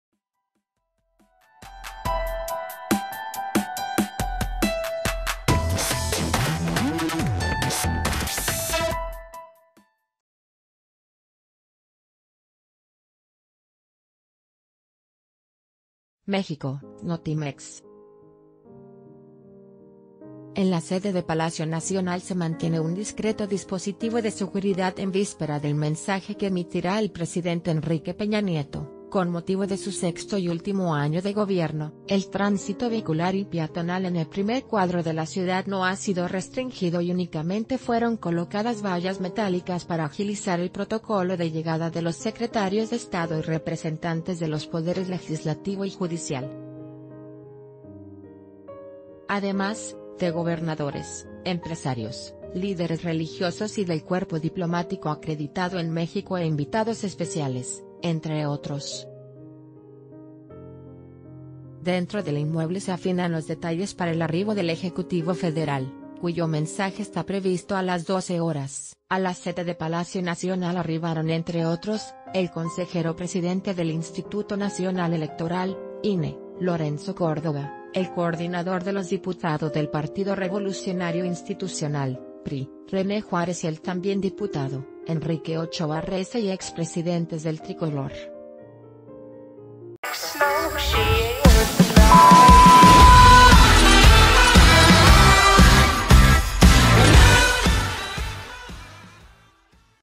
Peña Nieto ofrece su sexto informe en Palacio Nacional (En vivo) - El Diario de Yucatán
MÉXICO (Notimex). – En la sede de Palacio Nacional se mantiene un discreto dispositivo de seguridad en víspera del mensaje que emitirá el Presidente Enrique Peña Nieto, con motivo de su sexto y último año de gobierno. El tránsito vehicular y peatonal en el primer cuadro de la ciudad no ha sido restr...